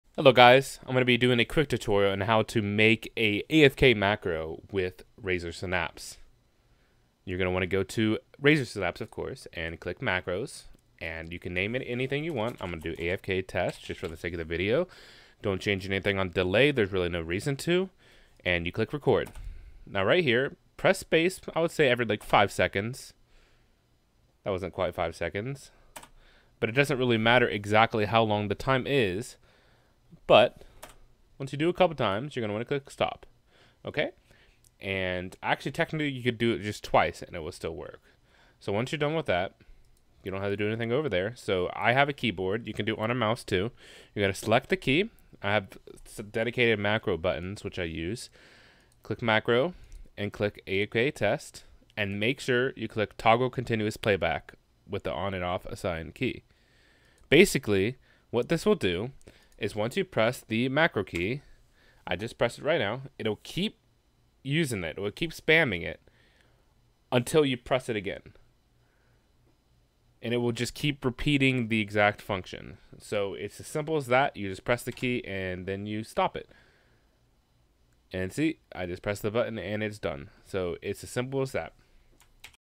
How to Make an AFK macro in Razer Synapse
The title says it all. Simple tutorial